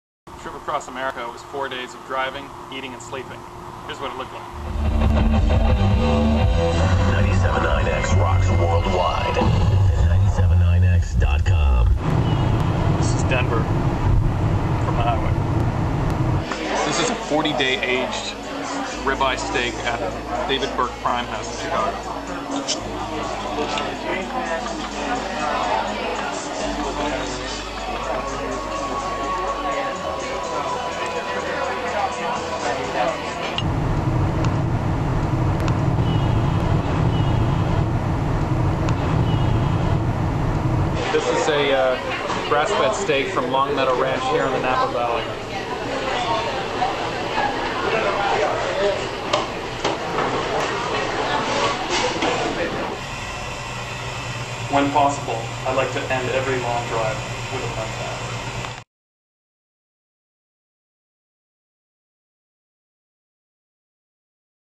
Concierge -- Mark Schatzker 80 Days Or Bust Travel Blog #3
Concierge.com asset, video.  Concierge.com travel writer, Mark Schatzker, treks around the world in 80 days.  Follow his intrepid journey on his blog as he attempts to circumnavigate the globe "the old-fashioned way."  Certain treats are acceptable, however, as he enjoys a fine ribeye steak at David Burke's in Denver and indulges in a mud bath in Napa Valley.  From Wiredset Digital Agency.